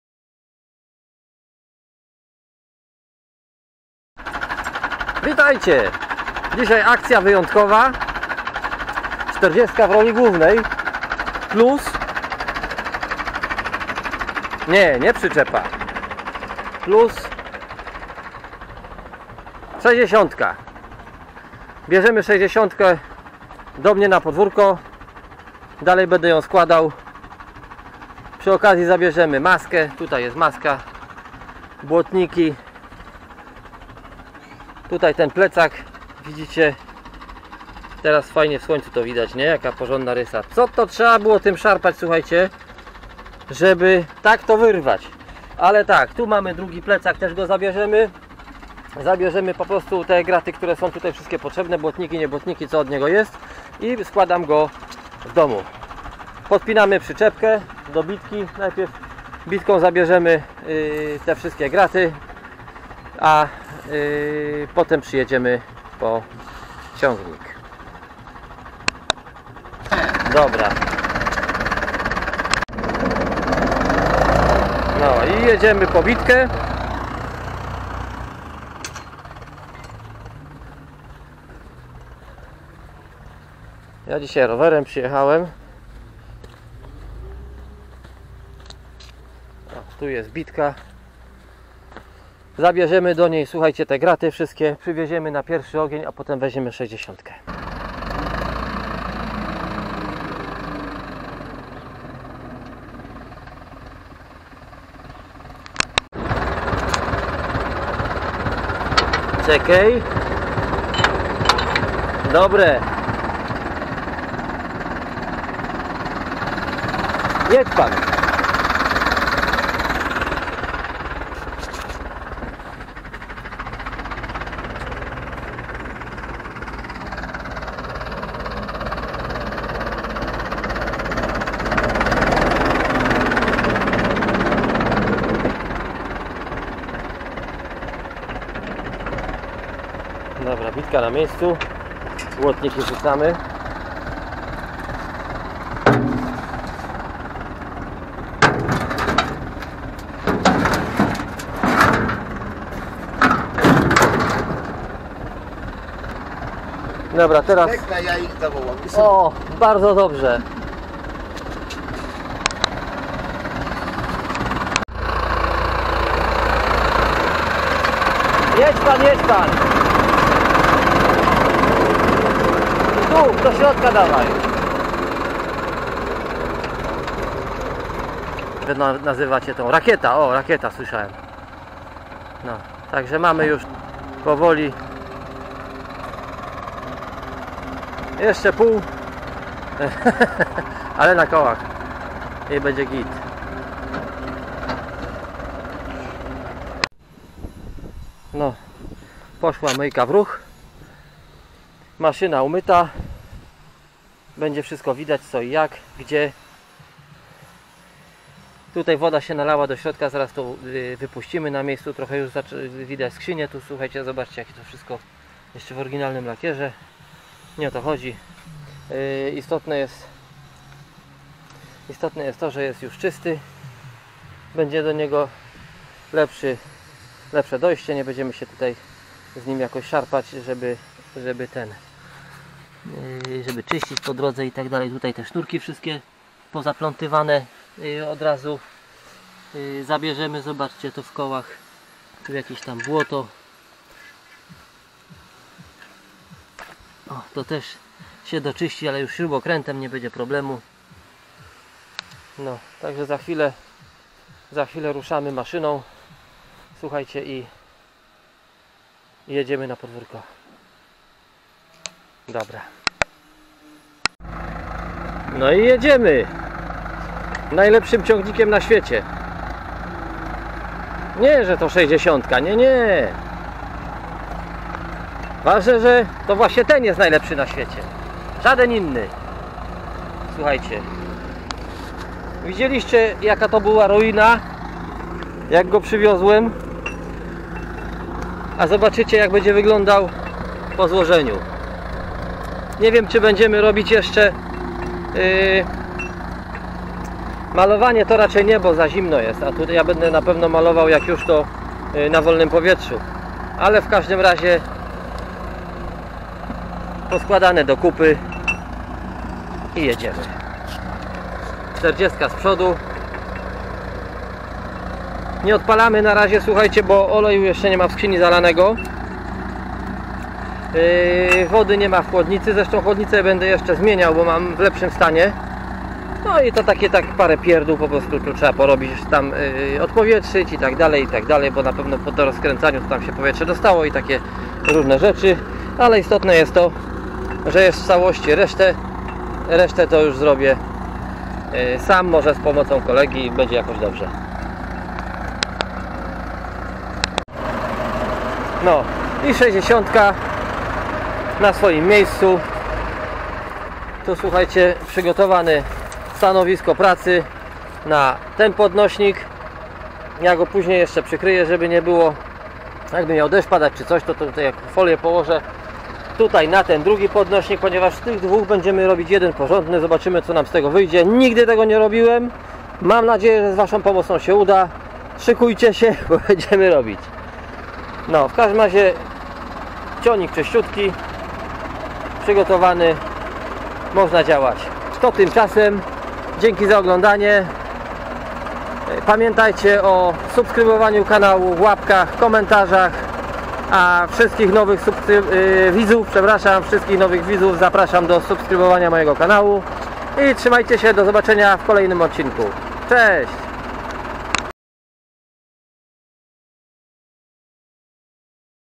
Najlepszy mechanik na świecie i Ursus C 360 - czerwona rakieta i gościnnie Ursus C 4011
@marekrolnik4699 Czerwona 60 tka, wraca na warsztat :)